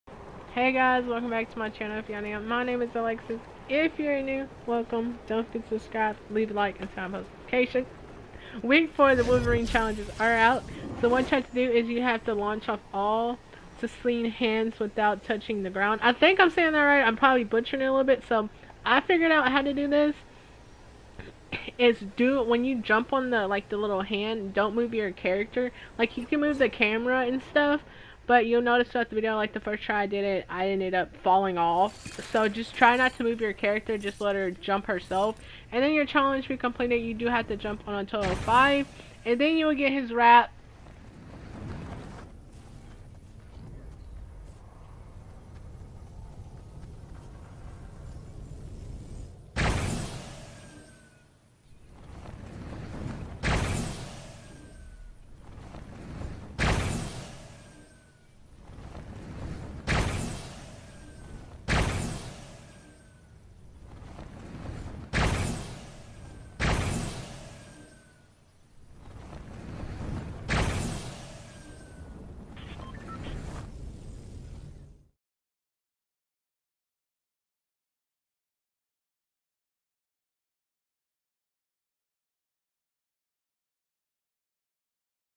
Wolverine Week 4 Challenge (Launch off all Sentinel hands without touching the ground) Season4
Hey cattotatoe💛

Wolverine Week 4 Challenge Launch off all Sentinel hands without touching the ground

Bye cattotatoe💛